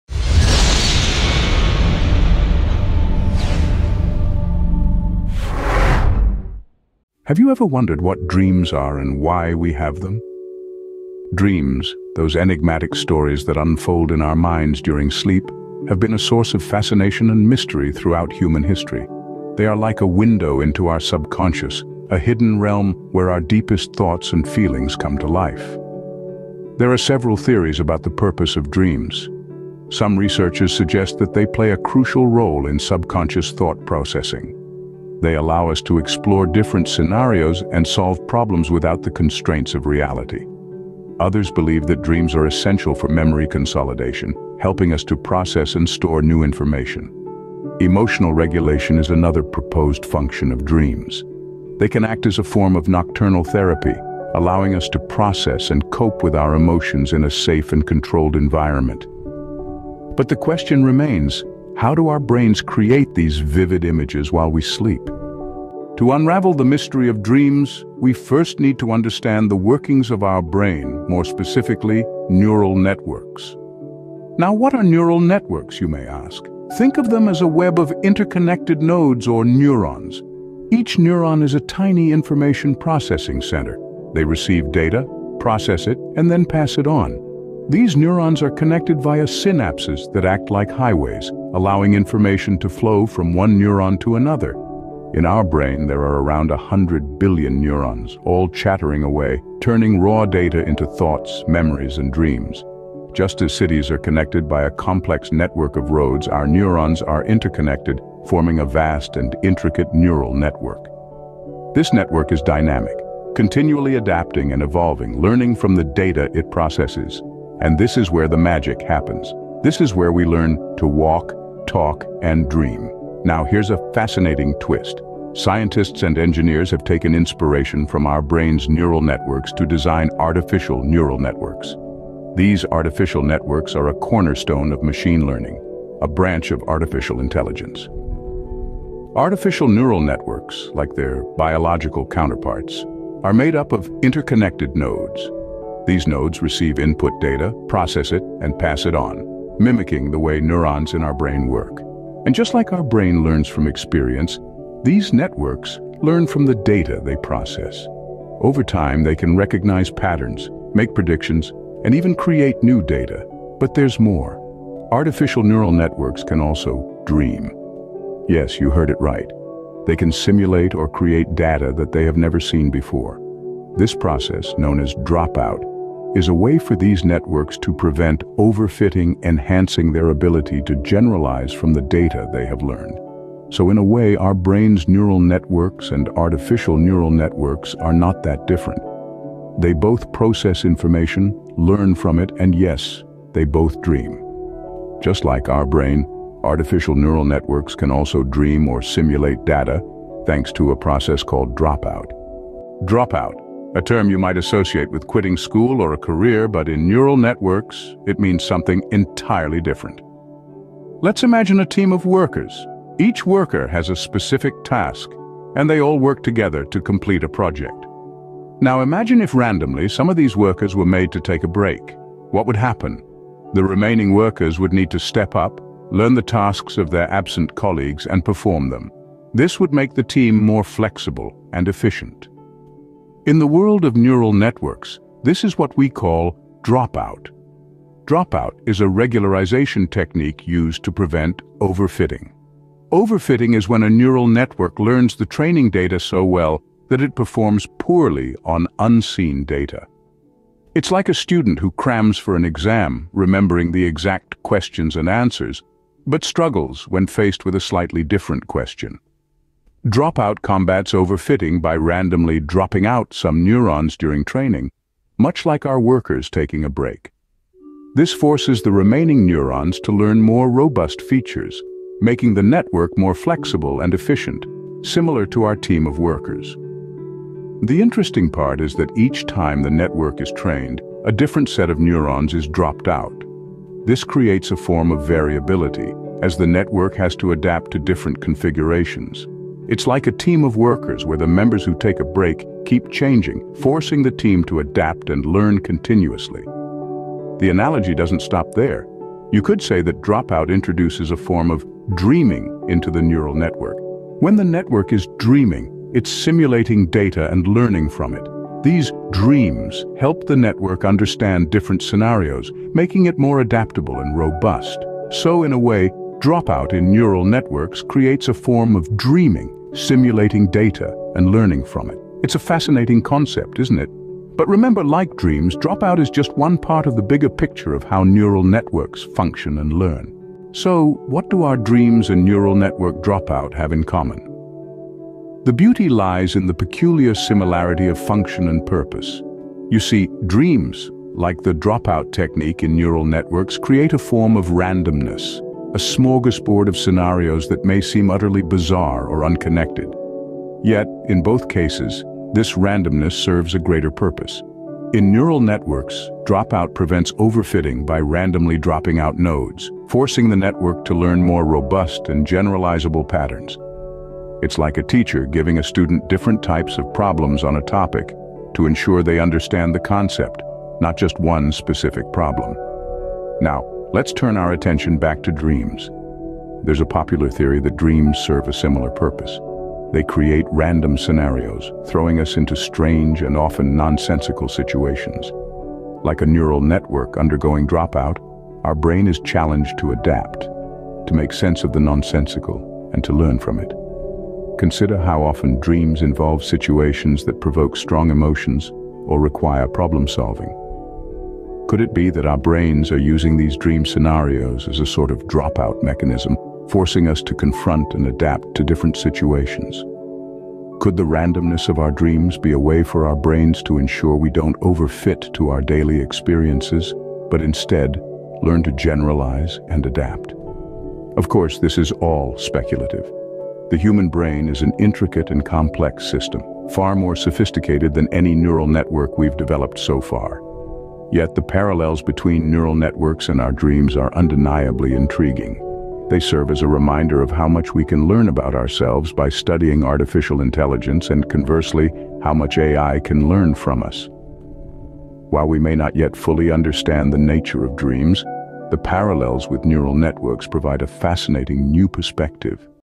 Secrets of Dreaming and Neural Network Dropout
Grow your youtube channel! Try VidIQ
Uncover the fascinating connection between dreaming and neural network dropout in 2024 in this insightful video. Join us as we delve into the science behind these intriguing phenomena and explore how they are linked. Don't miss out on this mind-expanding journey into the world of neuroscience and artificial intelligence. Watch now!